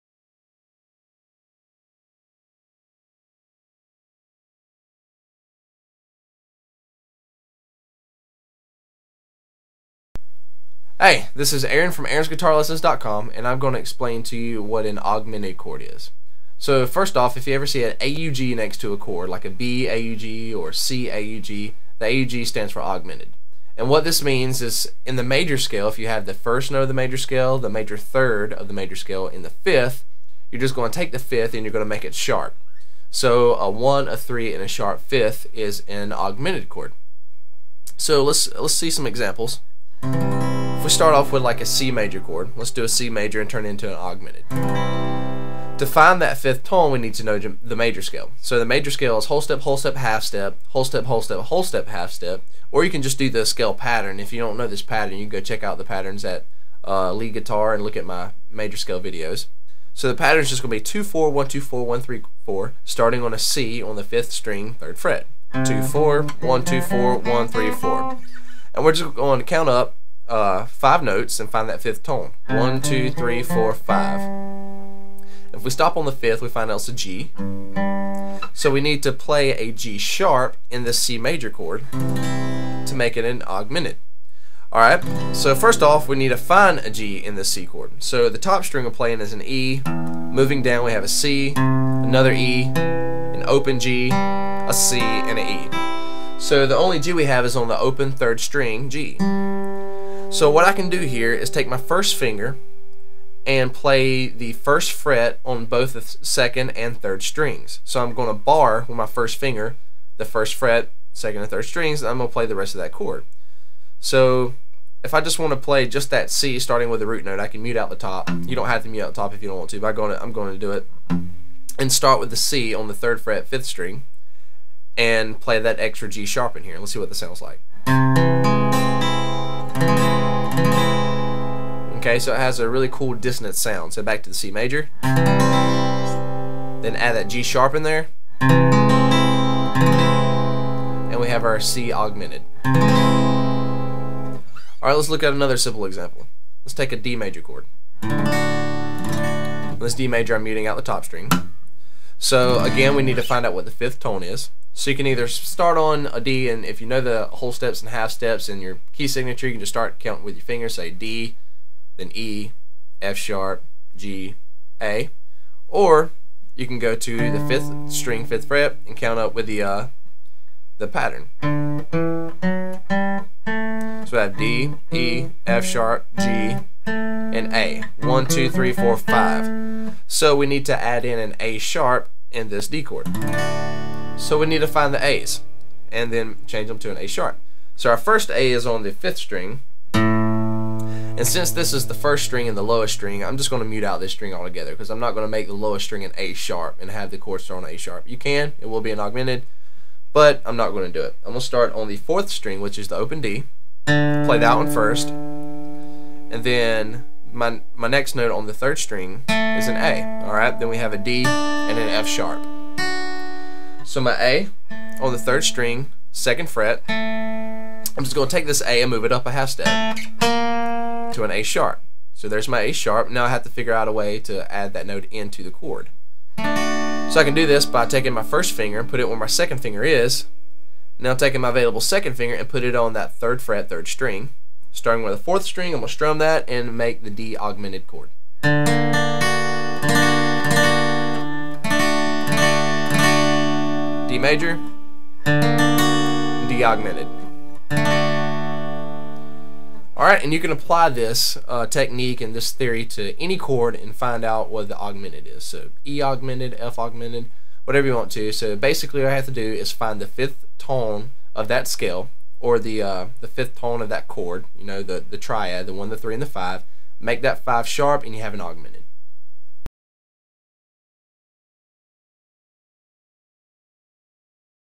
What is an Augmented chord
An augmented chord is simply the tonic, major third, and sharp fifth of any major scale.